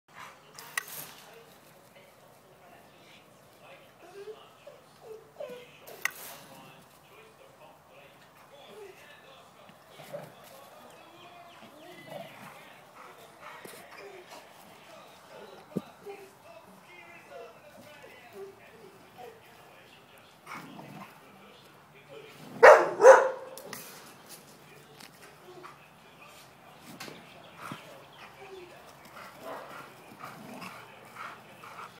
Cassie, Wolfhound X - looking for a home
Staffy Rescue dog Cassie looking for a forever home